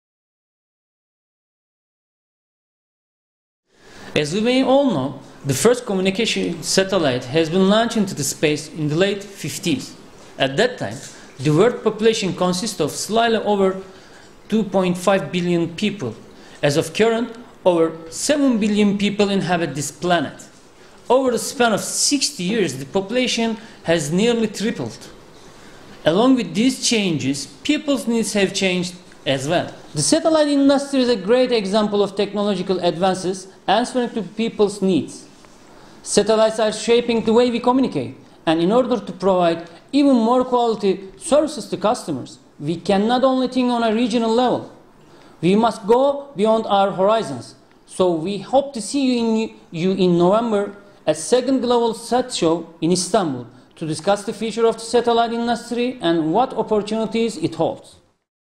Global Satshow CEO SUMMIT Video Message, Cenk ŞEN, CEO TÜRKSAT
Cenk ŞEN, CEO of TÜRKSAT, is going to speak at Global Satshow CEO Summit.

Come and Listen to his valuable opinions upon at “FUTURE COOPERATIONS BETWEEN GLOBAL AND REGIONAL SATELLITER OPERATORS” in 29-30 November, 2016 at Haliç Congress Center, ISTANBUL.